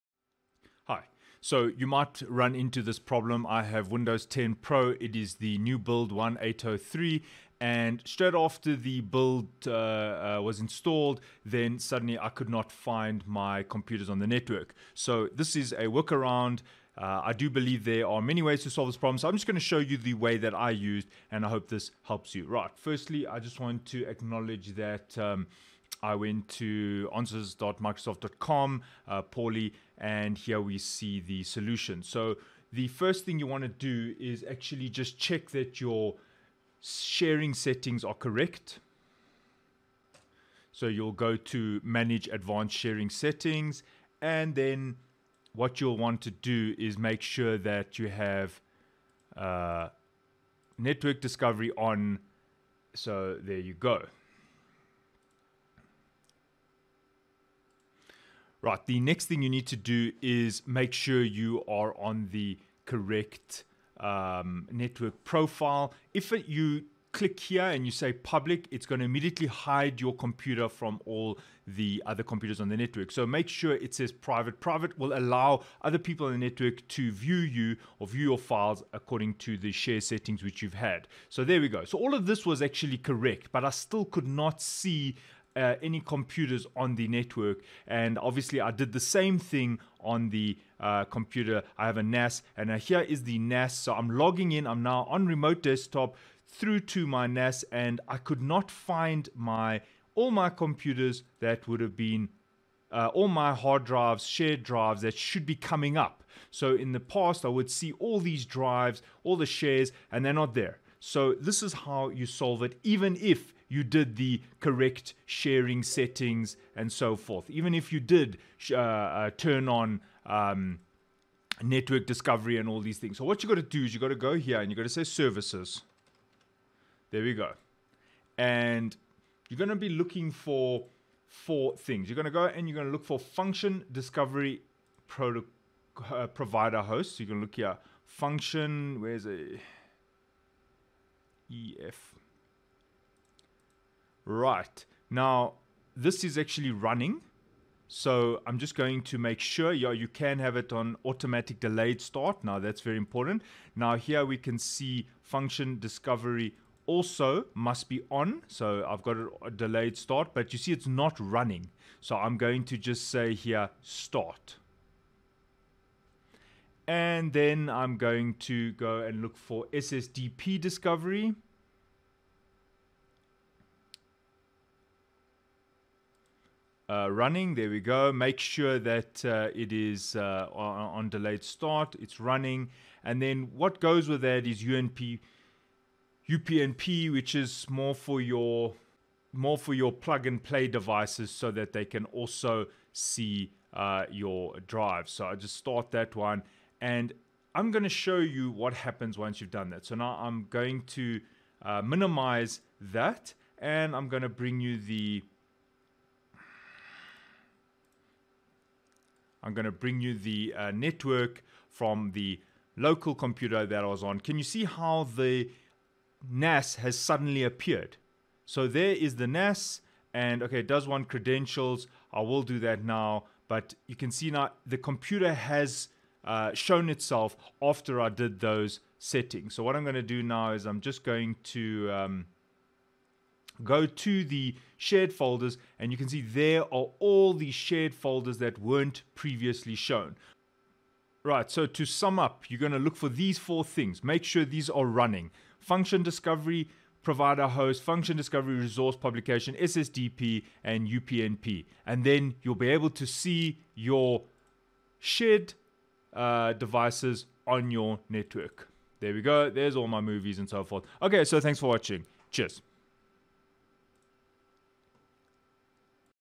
Can't see network computers after upgrade to v 1803 (Windows 10) - solved
Can't see network computers after upgrade to v 1803 (Windows 10)

Please re-share your folders if it still does not work